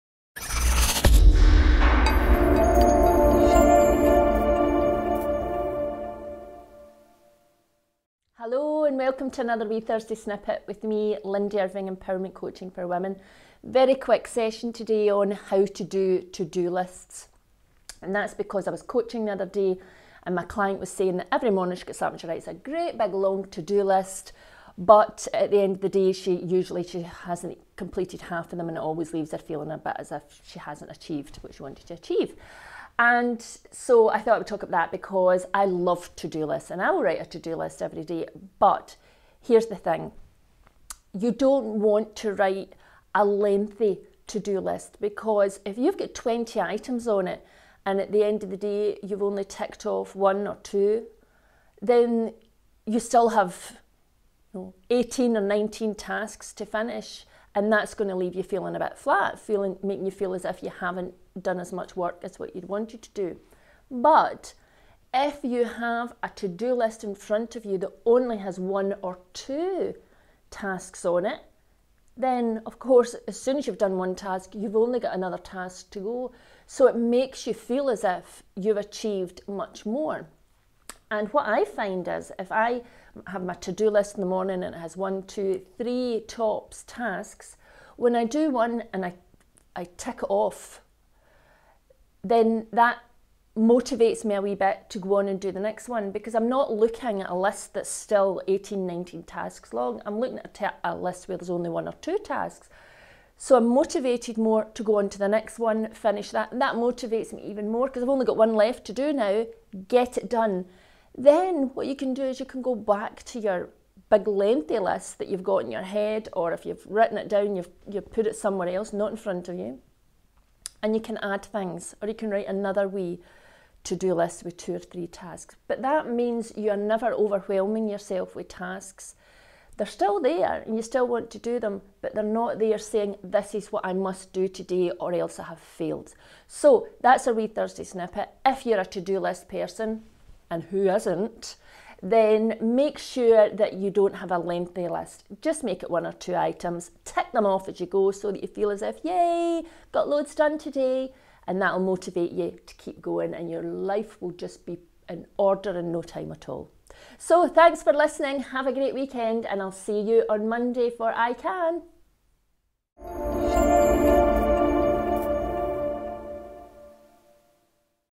How to Do a To Do List!
Are you someone who likes to write out a to do list but never seems to get through it? Then this short video is for you!